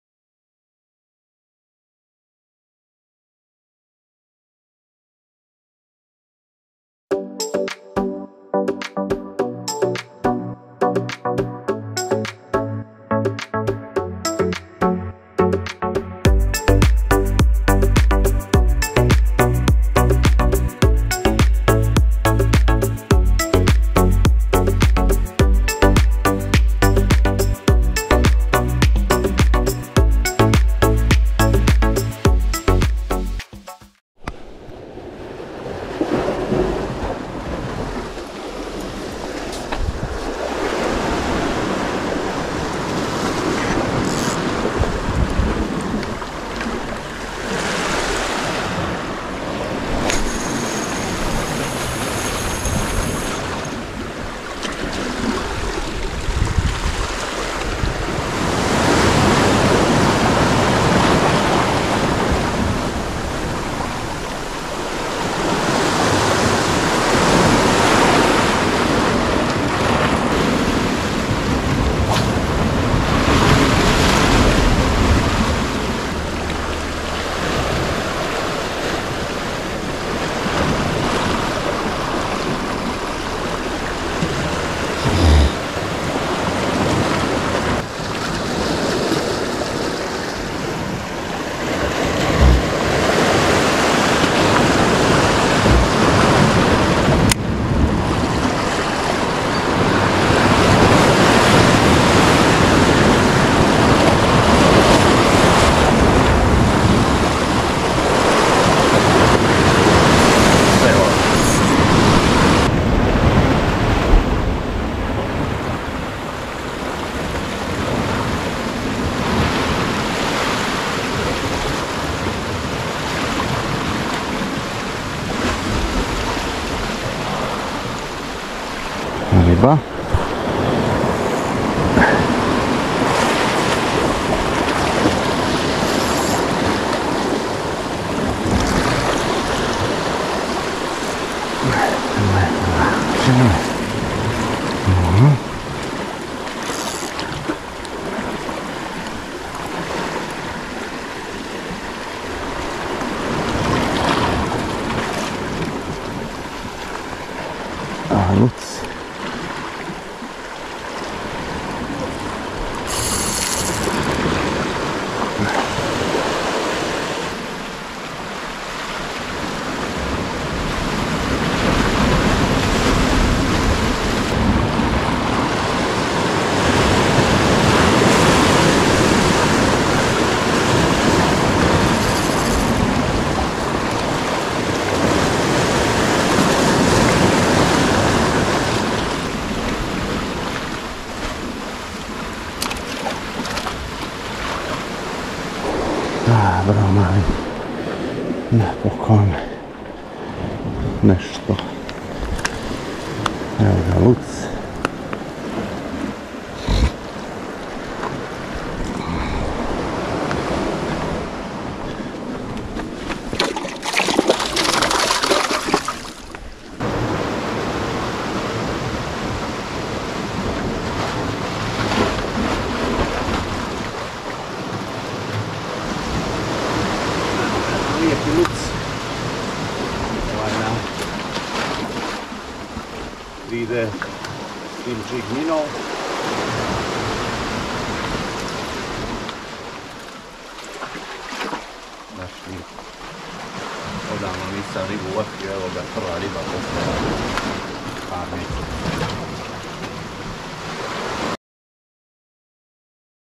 Shore Jigging - False Albacore in the morning
After few months of constant failures I finally catch a fish! Few months I didn't have a single strike and finally today I managed to catch an False Albacore on Savage Gear 3D Slim Jig Minnow 60gr in Pink decor. I hope I will keep uploading new videos and thanks all that was supporting me for a long time!

Equipment used:
Savage Gear SGS6 Shore Jigging 30-100gr
Savage Gear SGS10 6000 reel
Savage Gear 3D Slim Jig Minnow 60gr